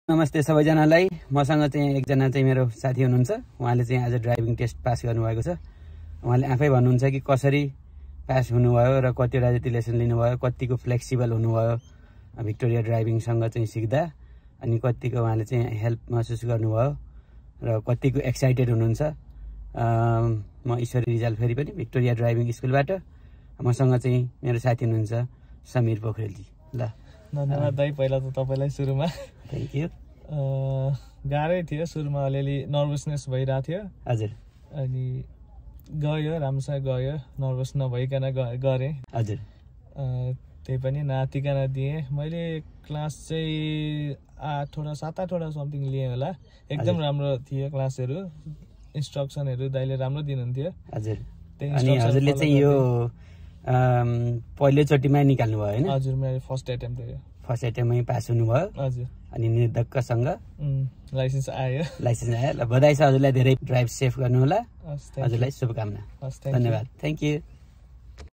Customer experience and feedback after passed driving test || customer satisfaction surveys.
Customer experience and feedback after passed the driving test || customer satisfaction surveys
This video is mentioning about experience
Driving test in the Melbourne
In nepali language ||